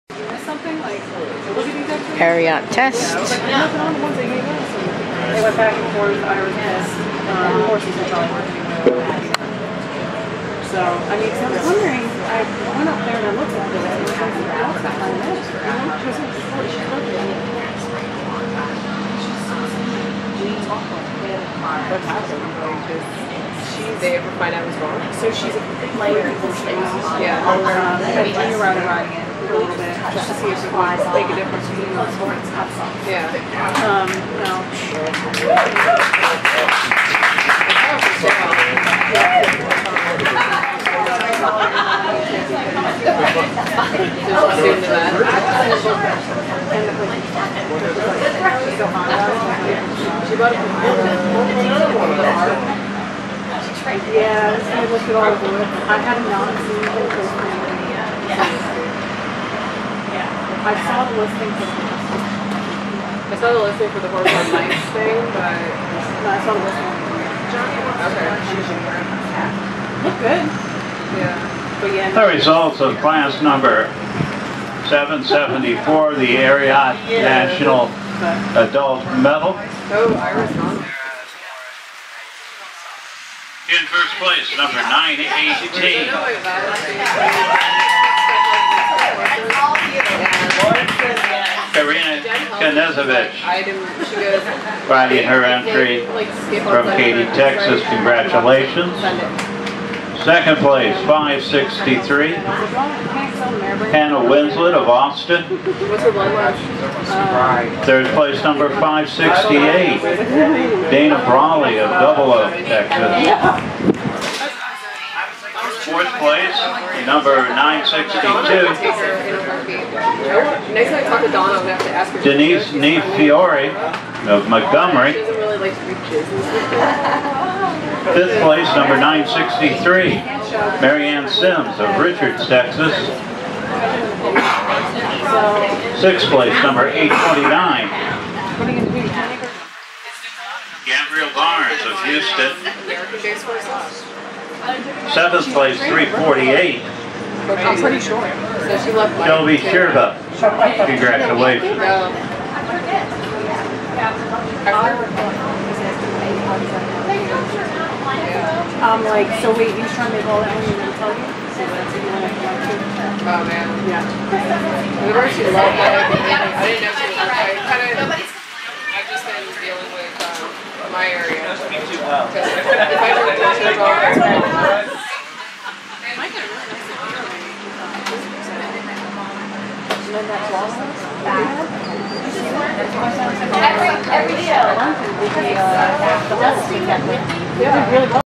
Video of LOVE AT FUERST SIGHT ridden by KARIANNA KNEZEVICH from ShowNet!
LOVE AT FUERST SIGHT ridden by KARIANNA KNEZEVICH at Pin Oak I 2017.  This round is from class 774, Ariat National Adult Medal and finished 5 of 7.